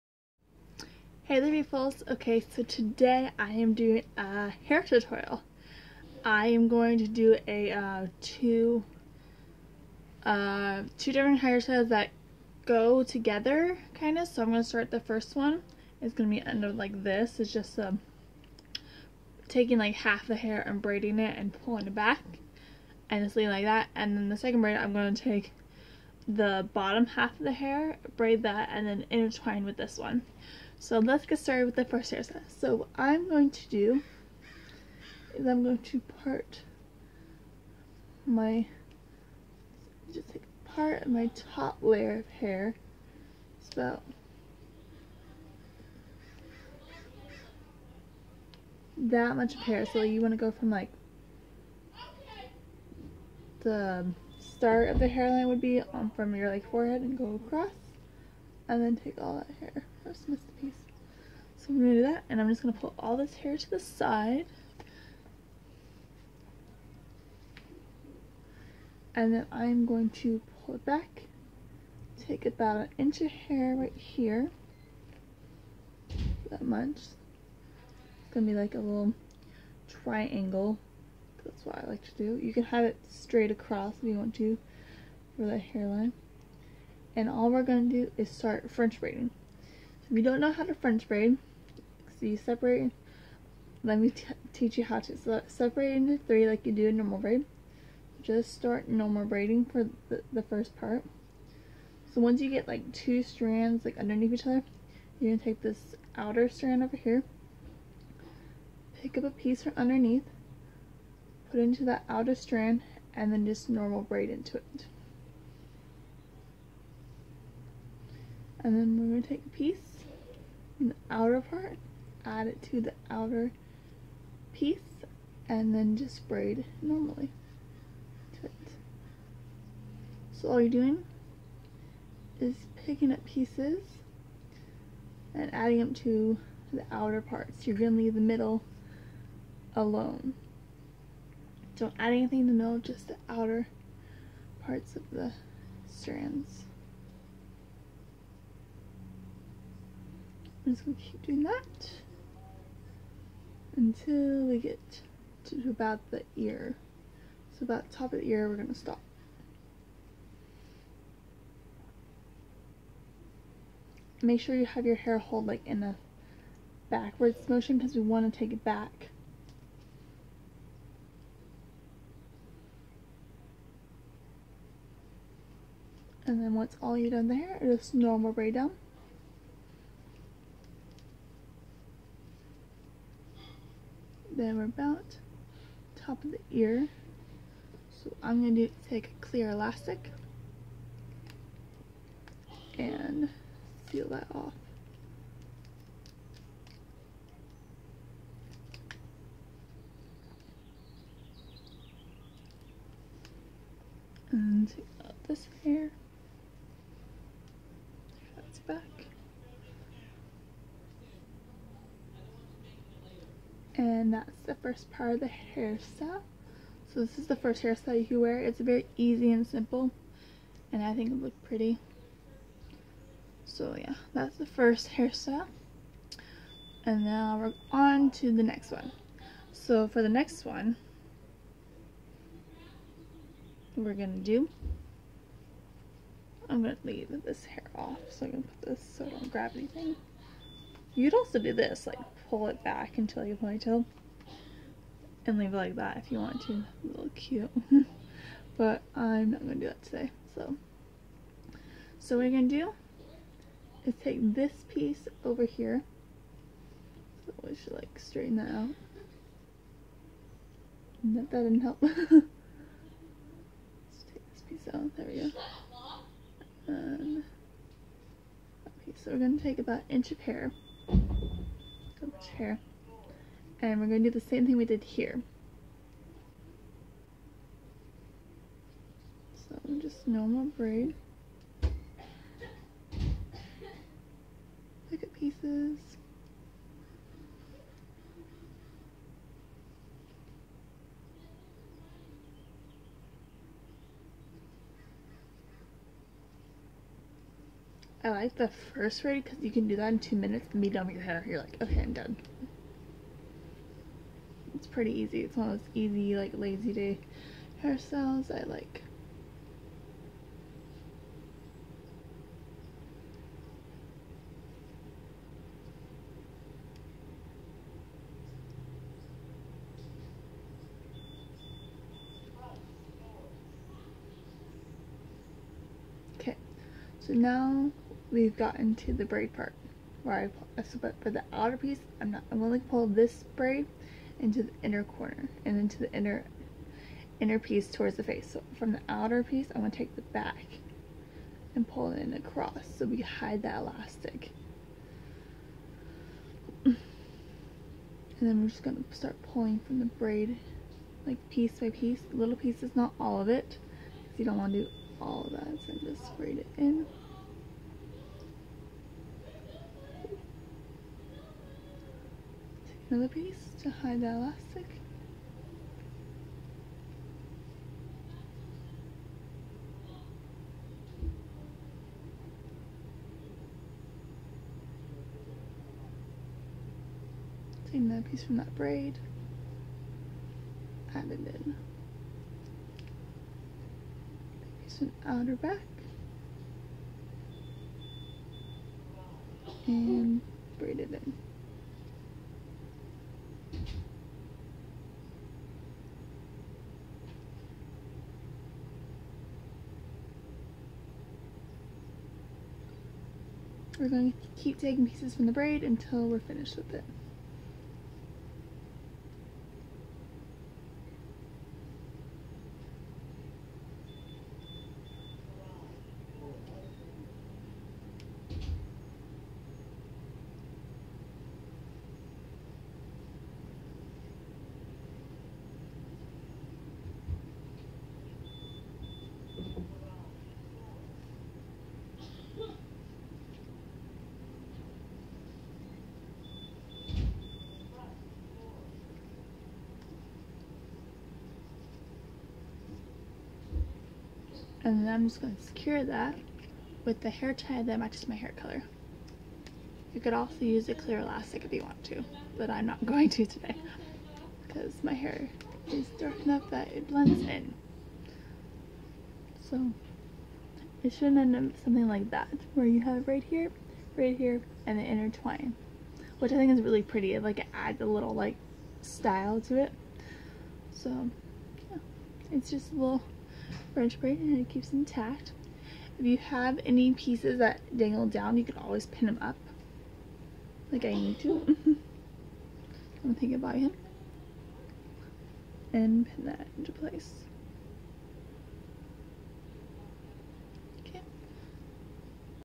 Double French Braid | YoutubeMakeup94
For today's special video, I showed you guys have to create a double french braid.